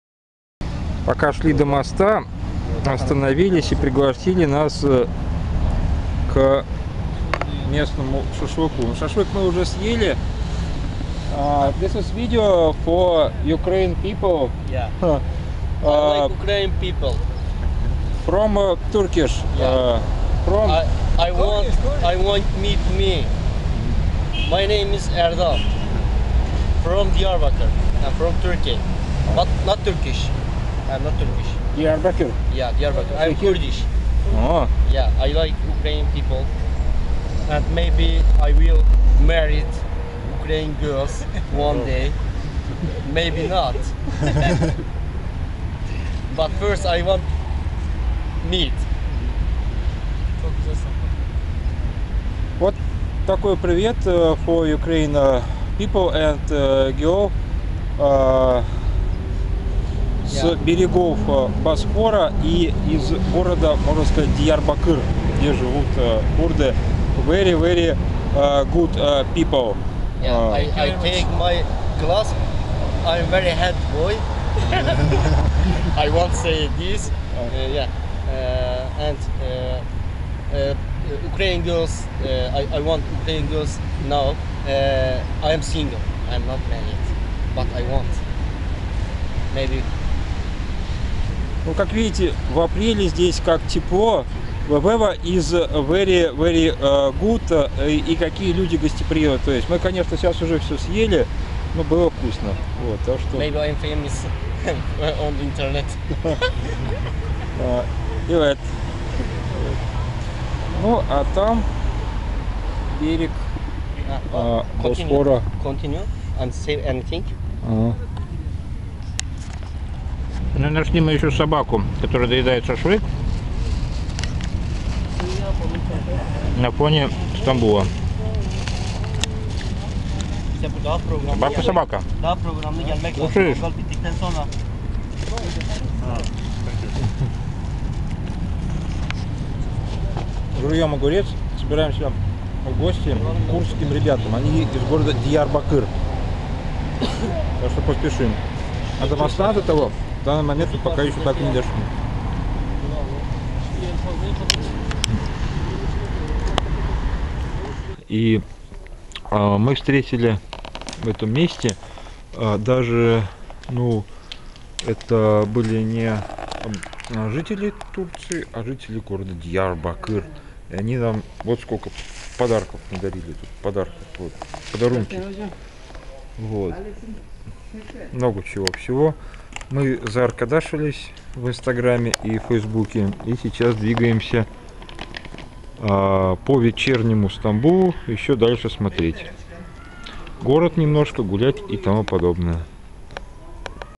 КУРДЫ ИЗ ДИЯРБАКЫРА об Украине и России. Стамбул-2016
Жители турецкого Курдистана передают привет в Украину! Ребята из города ДИЯРБАКЫР. Это город и район на юго-востоке Турции, на реке Тигр. Административный центр ила Диярбакыр. Неофициальная столица Турецкого Курдистана. Современное население города оценивается в 843 500 жителей, в основном курды (2010). Помимо курдов в городе проживают — турки, арабы, армяне, ассирийцы.
Диярбакыр является одним из древнейших городов мира. Ученые утверждают, что по некоторым источникам известно, что поселение на месте города существовало еще 7,5 тысяч лет назад. Теперь это место фигурирует как неофициальная столица турецкого Курдистана, хранящая память о множестве империй и сохранившая крепостную стену. Именно стена стала настоящим символом древнего города.

УЗНАЙ ЕЩЕ ПРЯМО СЕЙЧАС. Жми!